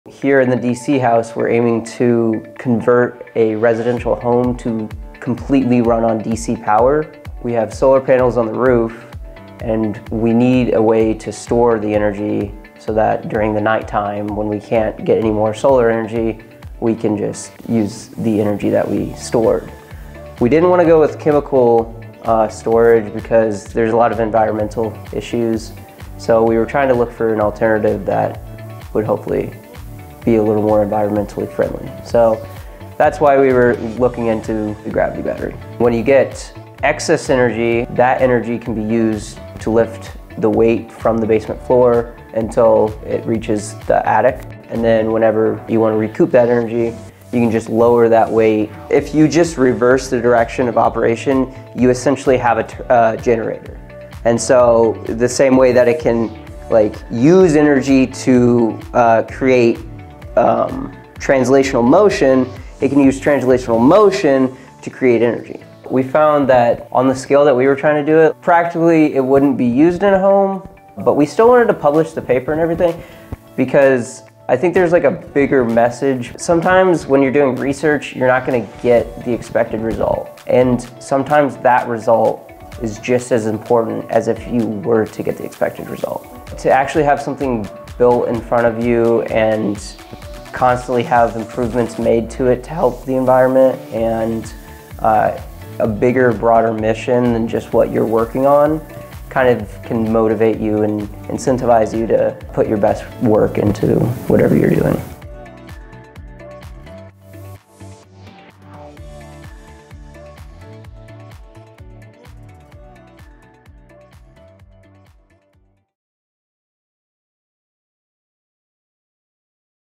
Could a “gravity battery” work in a residential home?
Sustainable energy sources like wind and solar present a challenge: how do you store excess energy during periods of overproduction for when you really need it? Some large-scale utilities have turned to mechanical energy storage: lifting heavy weights or pumping water uphill into a reservoir. Once that energy is needed, the weight is released and its mechanical energy powers a generator that produces electricity. These mechanical batteries have been proven to work on a large scale, but never at the scale of a single residential home. A team of Purdue University undergraduates undertook a study to investigate whether a “gravity battery” could be made small enough to fit into a single-family home.